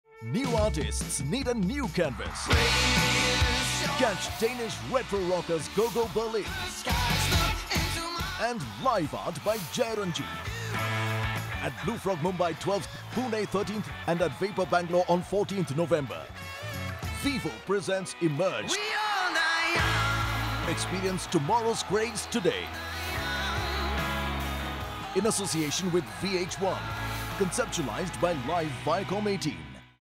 EMERGE: Go Go Berlin in Concert!
Go Go Berlin is coming LIVE in concert for the first time in India!

Also featuring LIVE art by Jai Ranjit!

Catch the unforgettable experience here:

12th November - BlueFROG, Mumbai
13th November - BlueFROG, Pune
14th November - Vapour, Bangalore

 A ROCKIN' night, you don't want to miss!